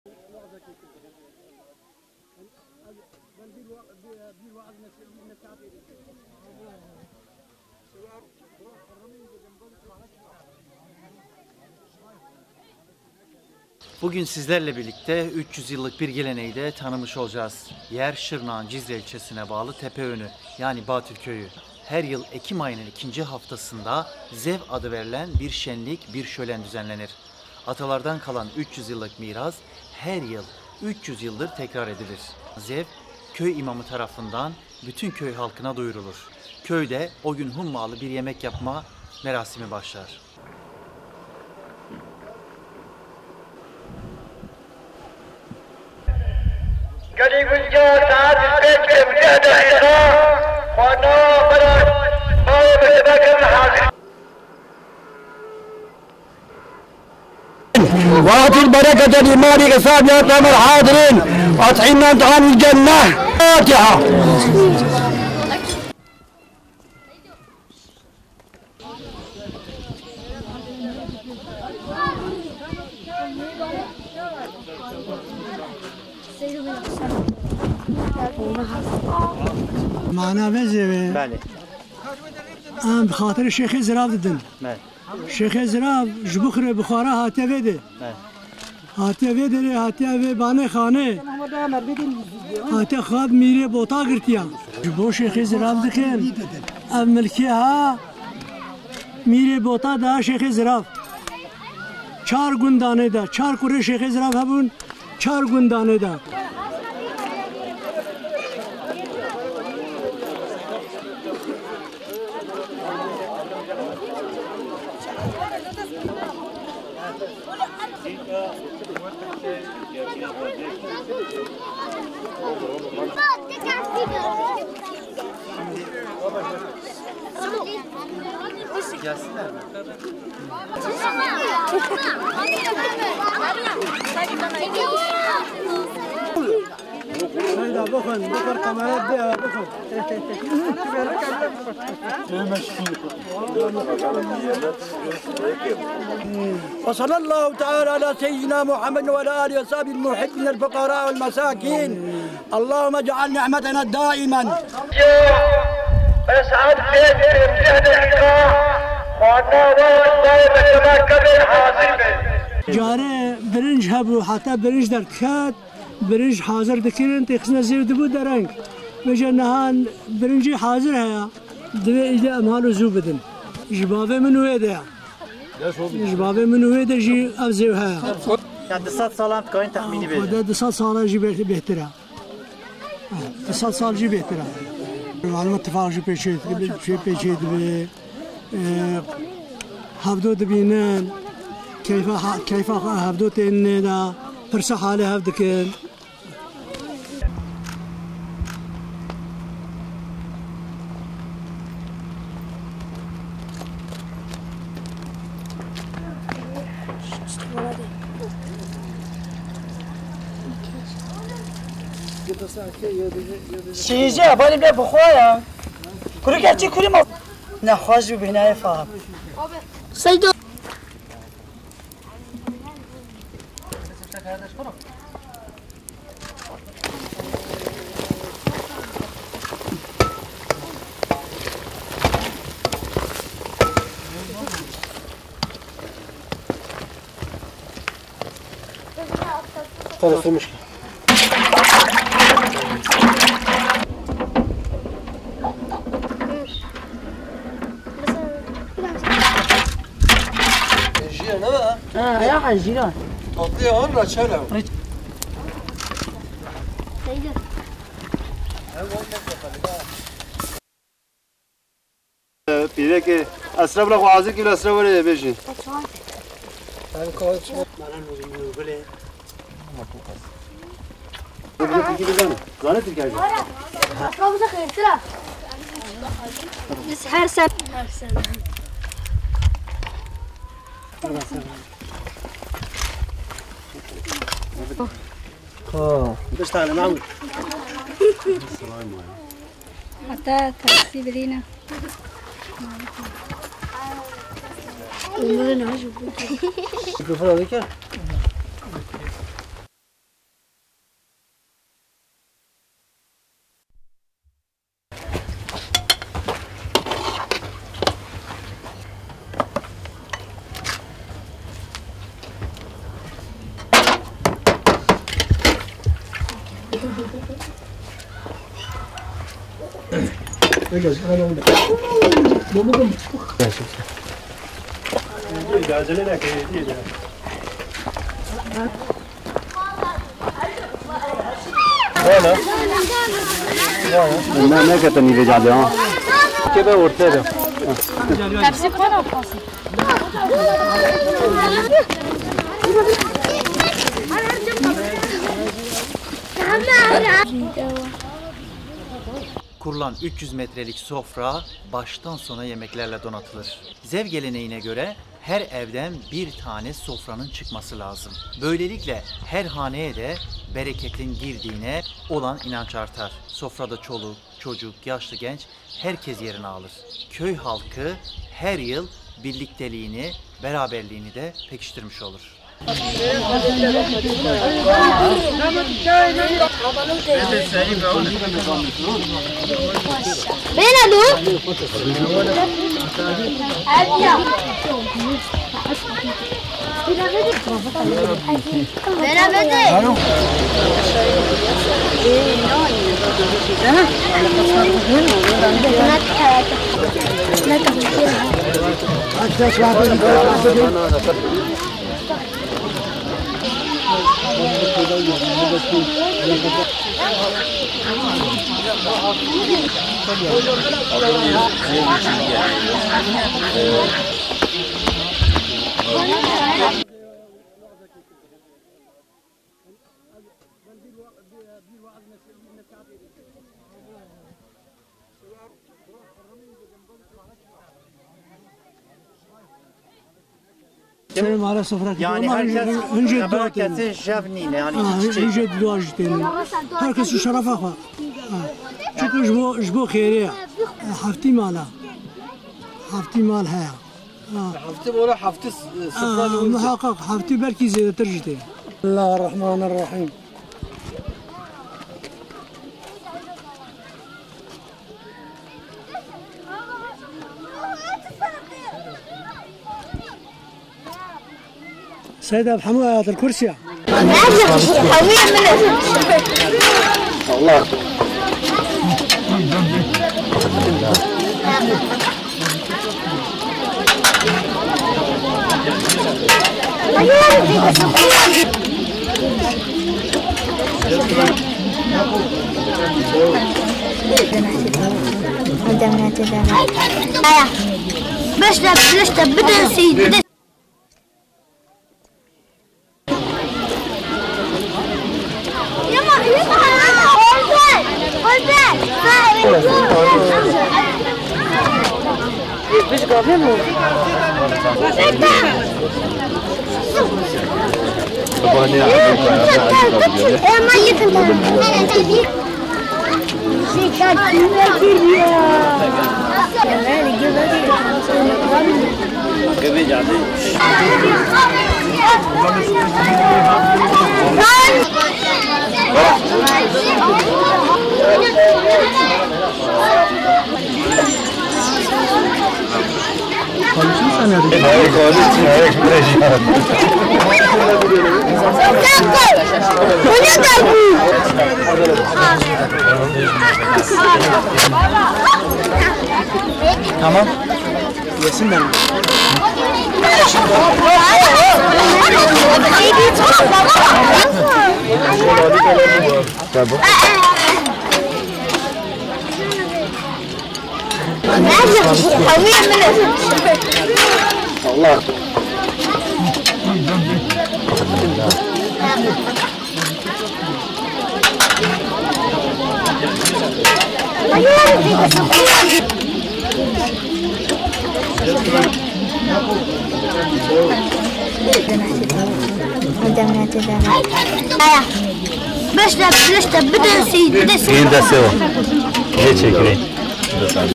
300 yıllık gelenek/köy meydanı sofrası/botanda gelenek ve görenekler/zew[gundêbatılê]
Şırnak’ın Cizre ilçesinin Tepeönü köyünde asırlık “zev” geleneği bugünde yaşatılıyor. “Zev” Kürtçe’de şükür ve bereket anlamına geliyor. Yöre insanı, her sonbaharda, köy meydanında hazırladıkları yer sofrasında buluşuyor. Bu buluşma, birlik, beraberlik ve dayanışmayı simgeliyor.(Zêwa Şêxê Zırav)